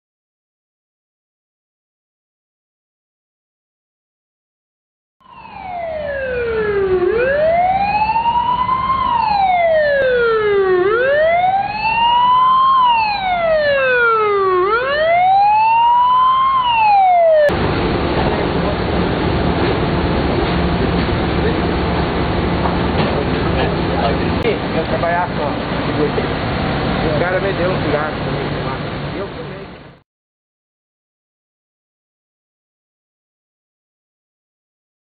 MS Record.com.br - PF de Dourados incinera mais de 14 toneladas de drogas em Ponta Porã
A Polícia Federal (PF) de Dourados realizou uma grande incineração de drogas nesta terça (14) e quarta-feira (15) em Ponta Porã. Ao todo, foram mais de 14 toneladas de entorpecentes que foram apreendidos nos últimos meses na cidade de Dourados e região.